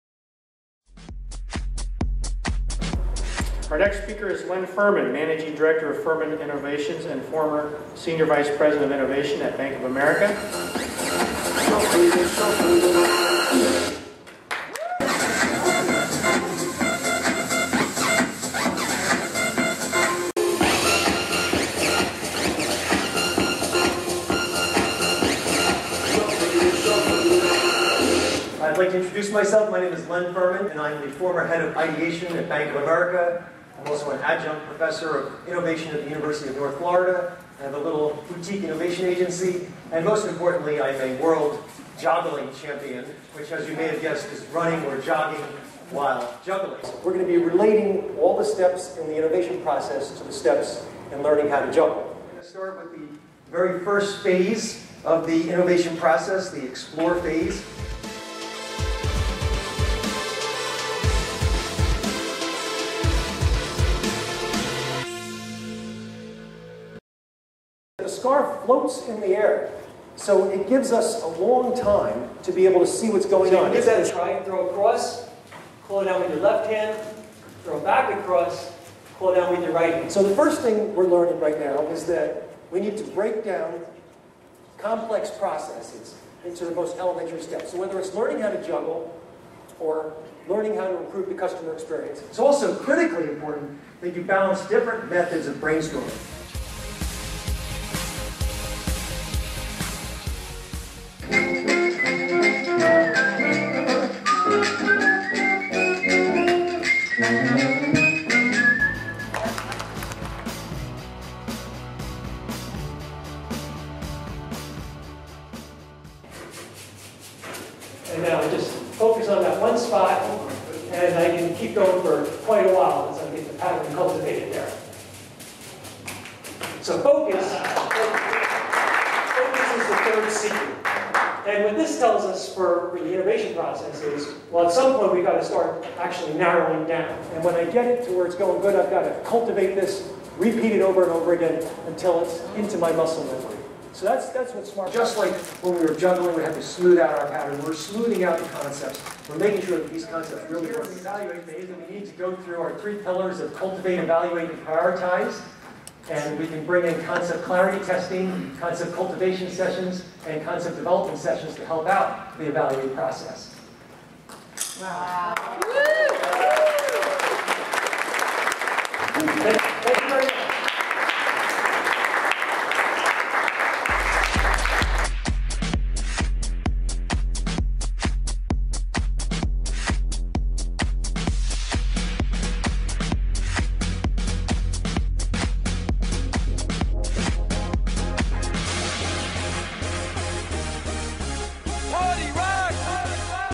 Len Ferman - INNOVATION SPEAKER
Energize your audience with a speaking program that will leave everyone inspired, informed and asking for more.
Len Ferman is the former head of ideation at Bank of America and a World "Joggling" Champion (running while juggling). He spent 25 years managing innovation at Fortune 100 companies prior to founding Ferman Innovation and becoming an adjunct professor of marketing at the University of North Florida.
Your entire audience will learn how to juggle (literally!). Len relates his business messages to each step in the process of learning to juggle.